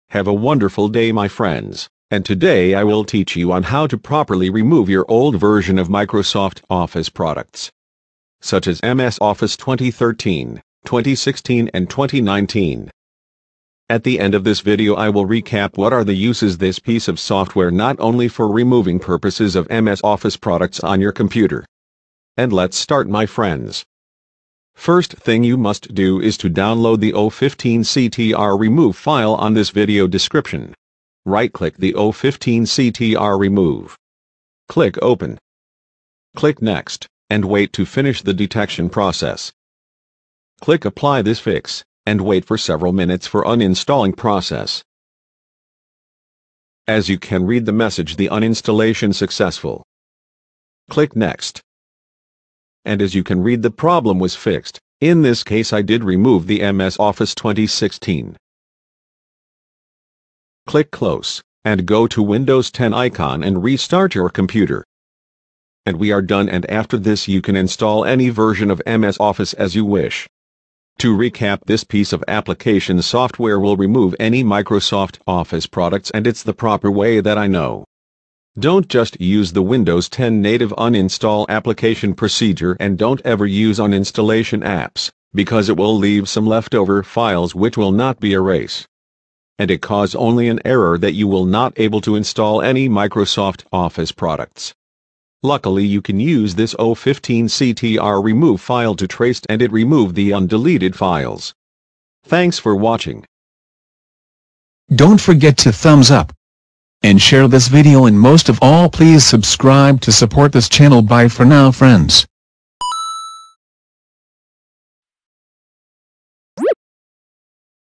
Properly Remove And Fix Install Microsoft Office Products
I will teach on how to properly remove and fix Install Microsoft Office Products.
DownLod link:
O15CTRRemove